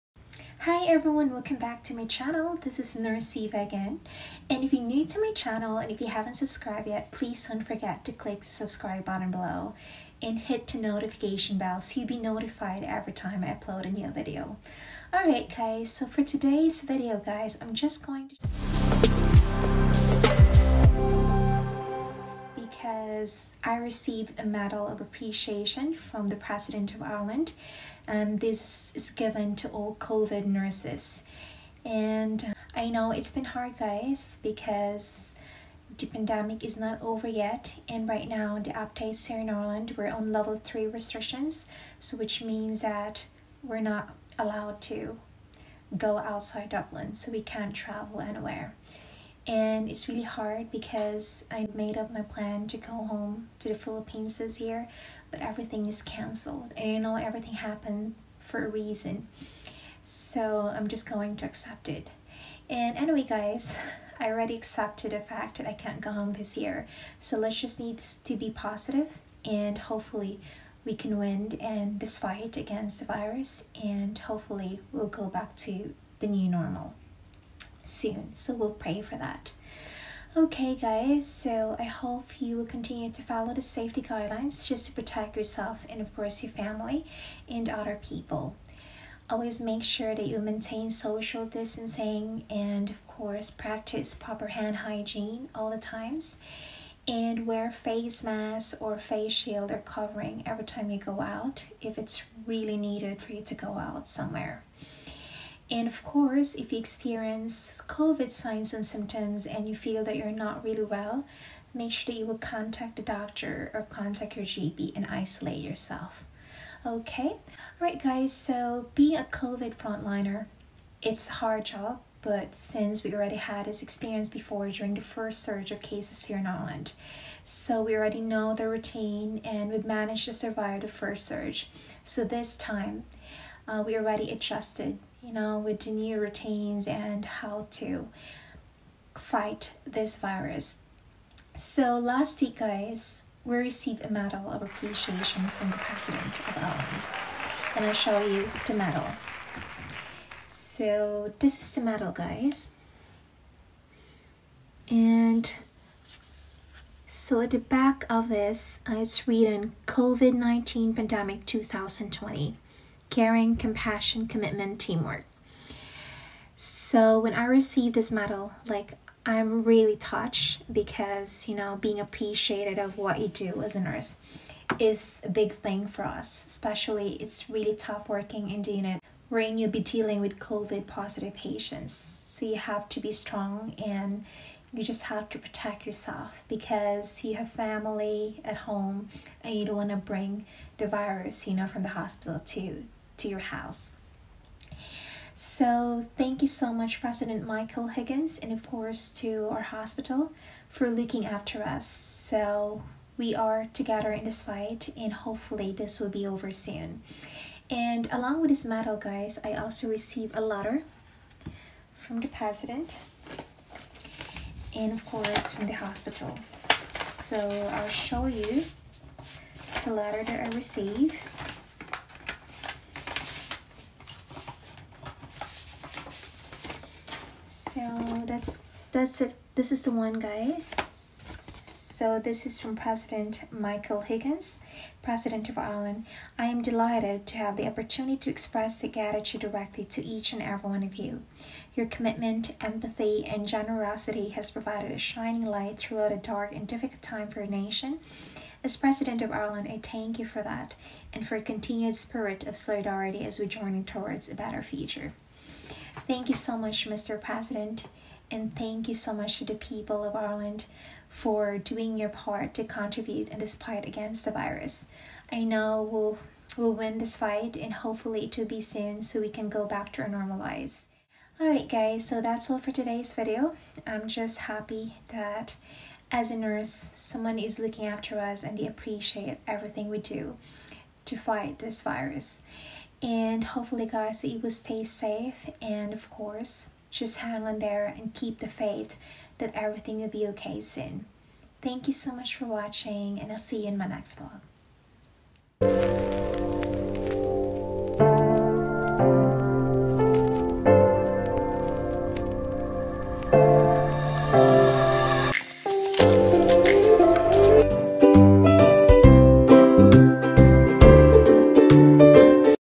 COVID-19 Frontliner||Medal from the President||Filipino Nurse in Ireland
As a COVID frontliner,I always have this fear everytime I go to work because everyone is vulnerable to get this virus.Every shift is tough but it’s always a privilege to be able to serve in the frontline.In addition,being appreciated in what we do means a lot.

Thank you President Higgins for this medal of appreciation to all frontliners and ofcourse to our Hospital for looking after us.This is something that we can show to our children and even grandchildren in the future.

Musician:LiQWYD